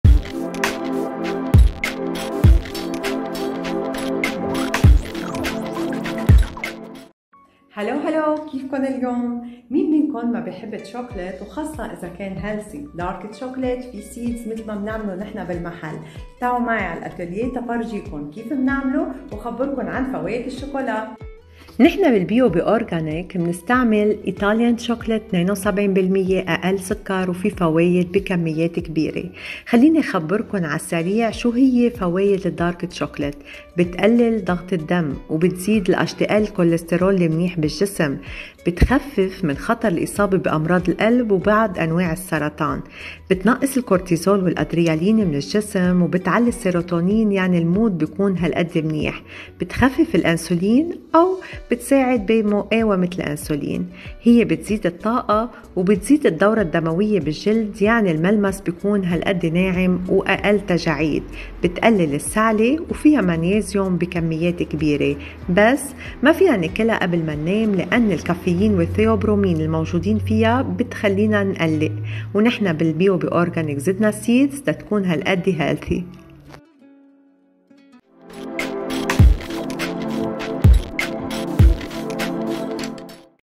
Dark chocolate with seeds - شوكولاتة داكنة مع البذور
كيف نصنع الشوكولاتة الداكنة الشهيرة بالبذور في متجر Bio Be Organic

برنامج خاص عن المنتوجات العضوية وفوائدها
مع كوزيت غالب

كل يوم إثنين على راديو برمانا

Radio Brummana
راديو برمانا الإذاعة الإلكترونيّة الأولى في لبنان
The 1st Online Radio in Lebanon